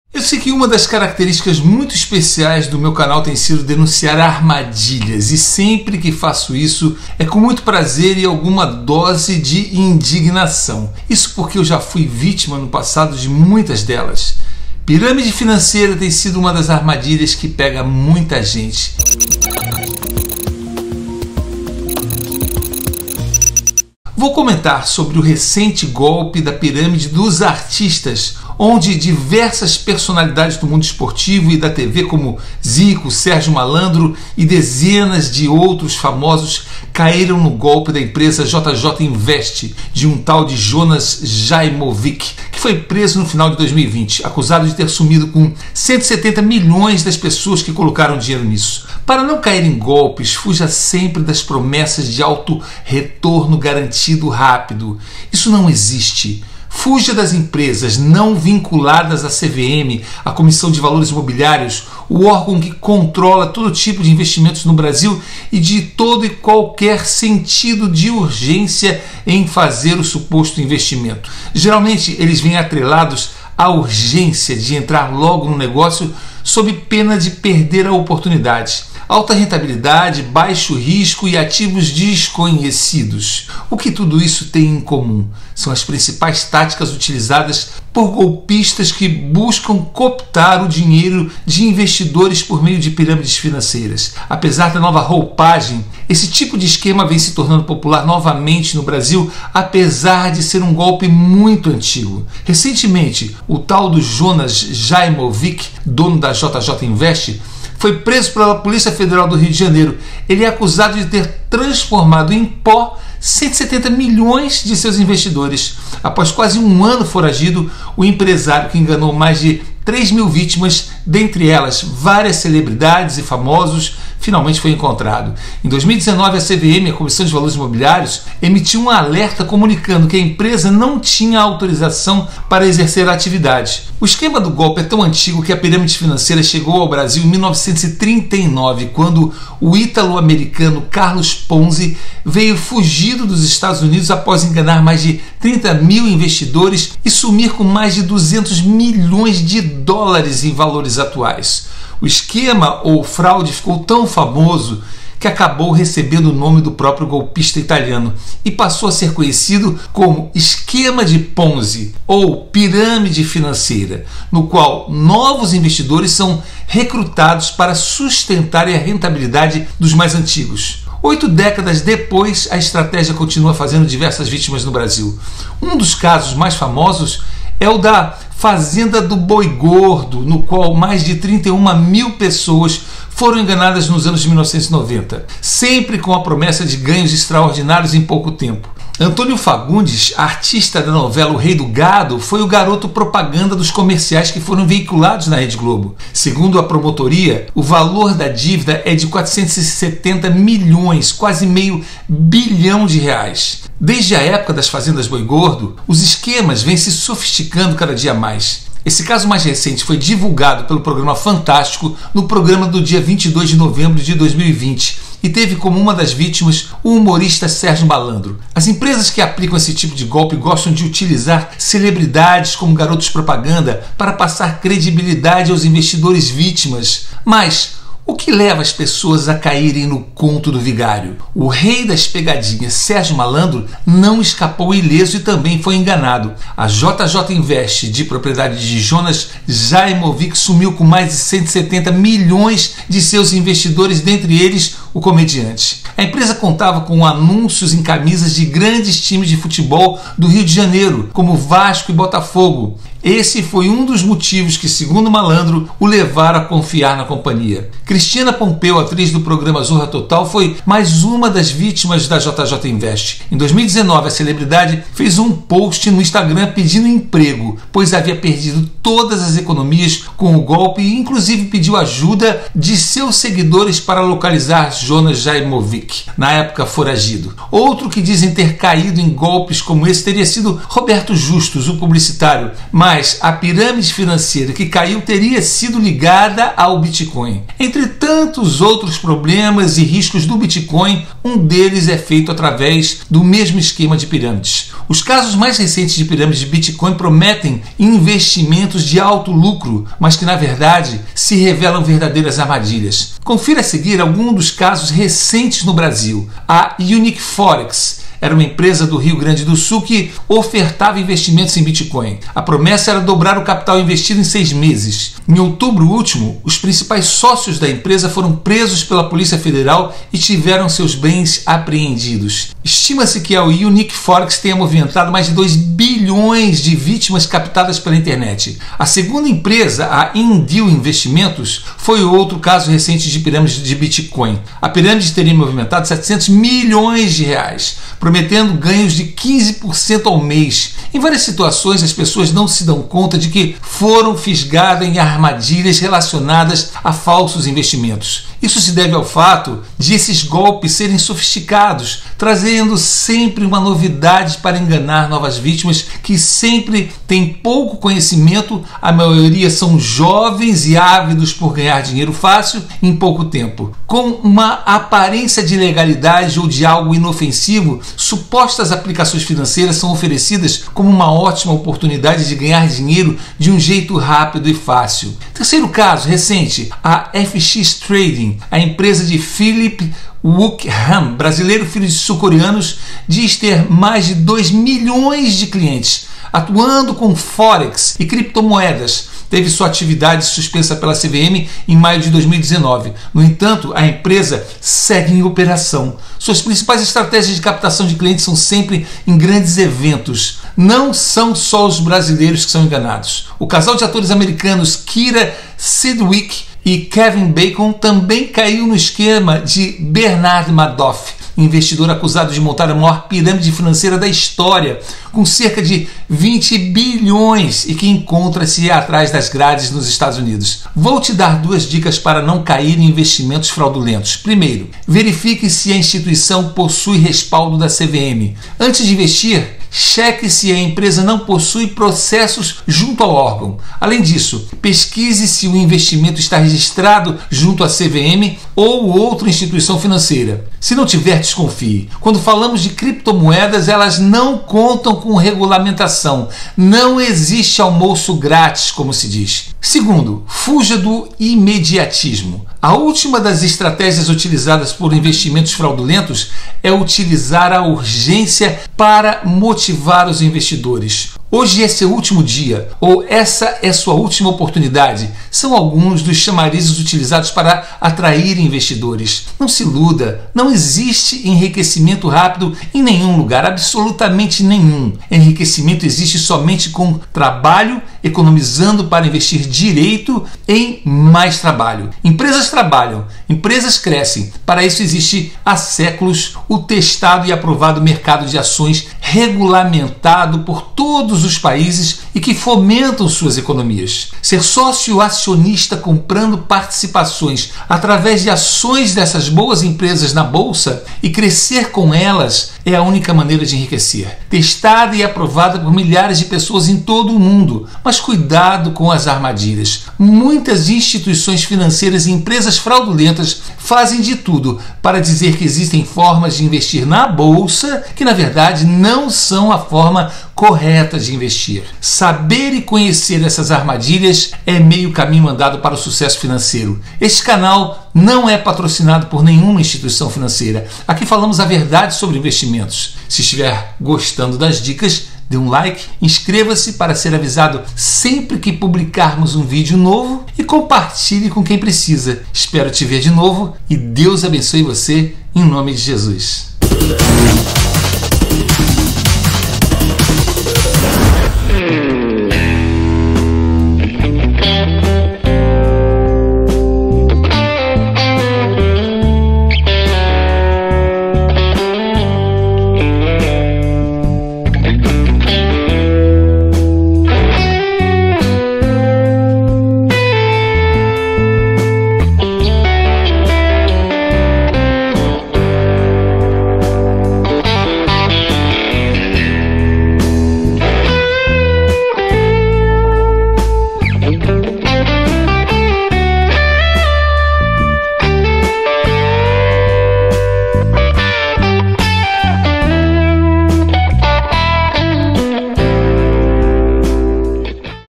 COMO ENRIQUECER
Eu sei que uma das características muito especiais do meu canal tem sido denunciar armadilhas, e sempre que faço isso, é com muito prazer e alguma boa dose de indignação, isso porque eu já fui vítima no passado de muitas delas. Pirâmide financeira tem sido uma das armadilhas que pega muita gente, infelizmente isso porque, as pessoas ainda caem na falsa promessa do enriquecimento rápido. Veja como funciona mais esse golpe: Para ilustrar o fato, vou comentar sobre o recente golpe da Pirâmide dos artistas onde diversa personalidades do mundo esportivo e da TV, como Zico, Sérgio Mallandro e dezenas de outros famosos, caíram no golpe da empresa JJ Invest de um tal de Jonas Jaimovick